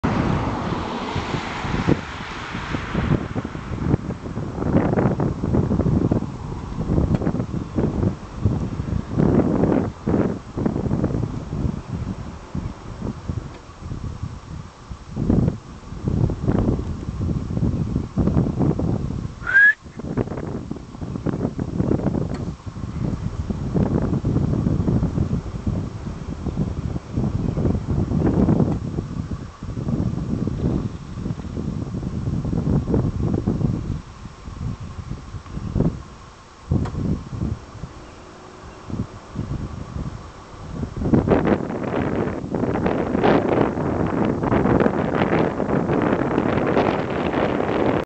Bull Moose eating near Minnesing MTB Trail
After crossing the highway the bull decided to nibble on the brush beside the Minnesing MTB Trail access road.